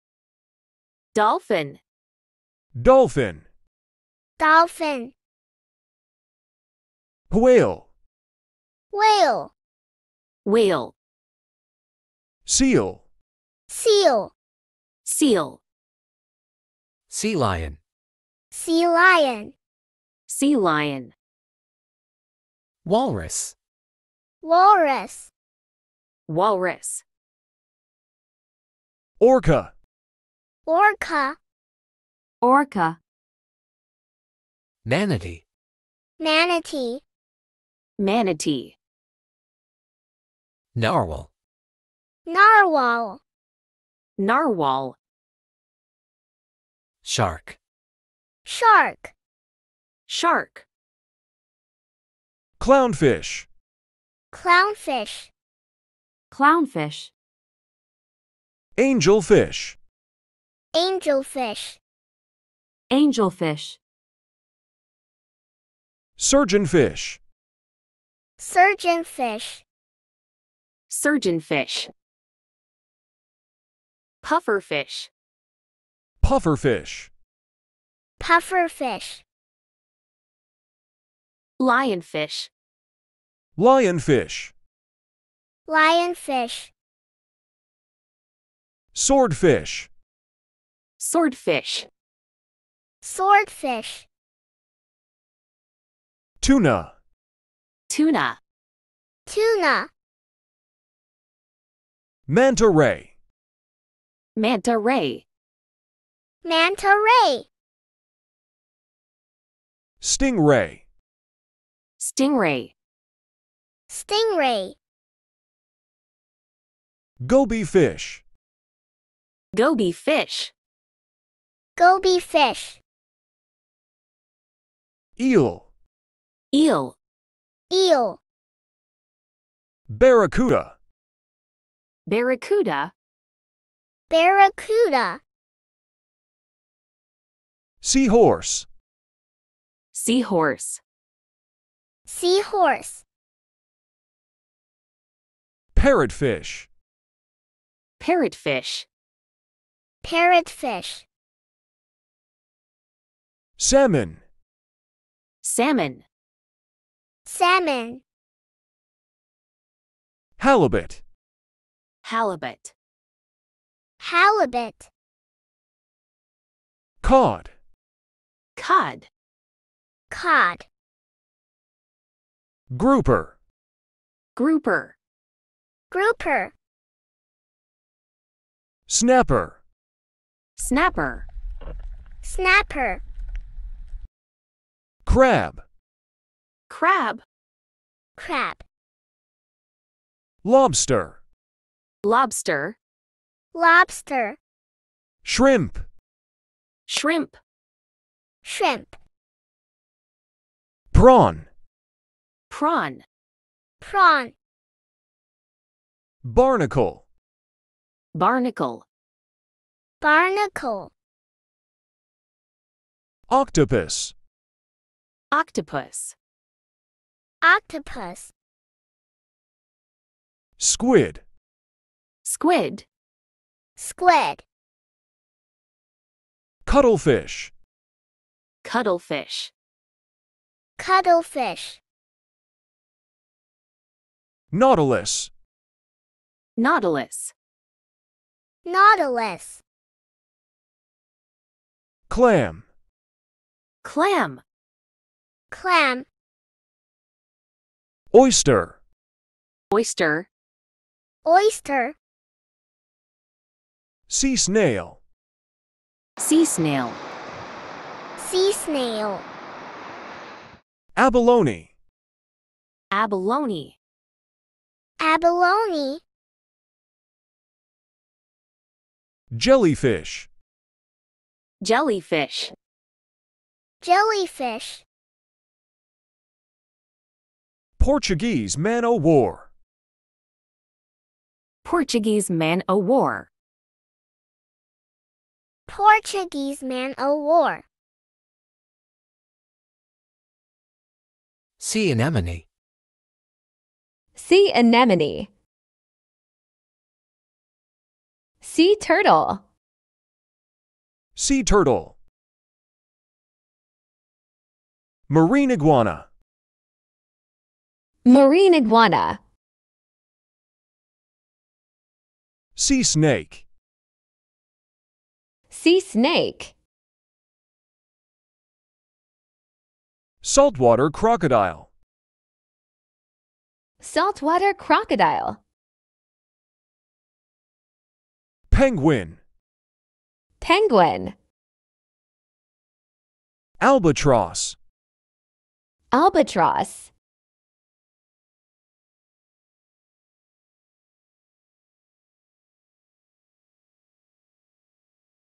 9 - Sea Animals Names Learn Vocabulary In English – Part 1
Sea Animals Names | Learn Vocabulary In English |– Part 1
🌊 Discover the Wonders of the Ocean! 🌊
In this video, we explore the 100 most popular sea animals from majestic whales and playful dolphins to vibrant clownfish and mysterious deep-sea creatures like the anglerfish. Whether you’re fascinated by sharks, sea turtles, or colorful coral reef inhabitants, this ultimate guide will take you on a virtual dive into the incredible diversity of marine life.
📖 Chapters:
0:01 - Mammals 🐋 (Whales, Dolphins, Seals, and more)
0:46 - Fish 🐟 (Sharks, Clownfish, Tuna, and more)
3:04 - Crustaceans 🦀 (Crabs, Lobsters, Shrimp, and more)
3:32 - Mollusks 🦑 (Octopus, Squid, Oysters, and more)
4:28 - Jelly-like Creatures 🌐 (Jellyfish, Sea Anemones, and more)
4:57 - Reptiles 🐢 (Sea Turtles, Sea Snakes, and more)

Perfect for nature enthusiasts, students, and anyone curious about the underwater world! 🐠🐙🐳